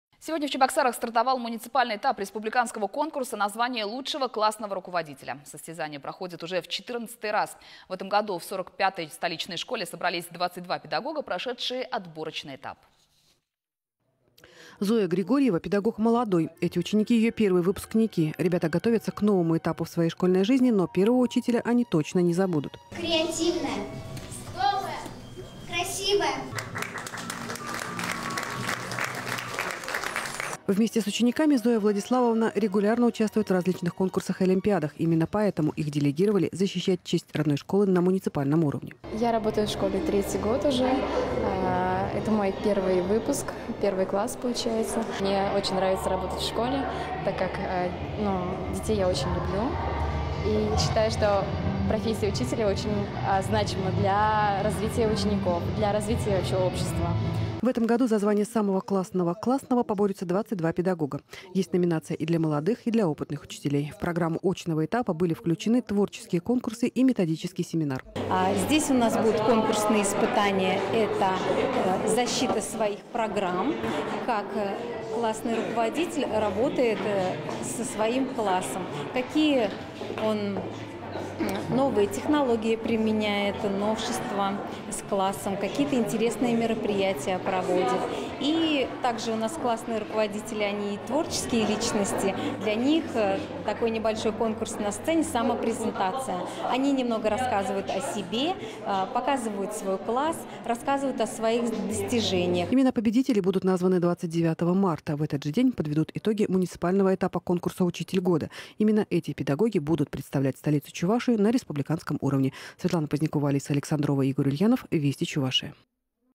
Педагоги Чувашии борются за звание «Самый классный классный»
Зоя Григорьева - педагог молодой. Эти ученики - её первые выпускники. Ребята готовятся к новому этапу в своей школьной жизни, но первого учителя они точно не забудут.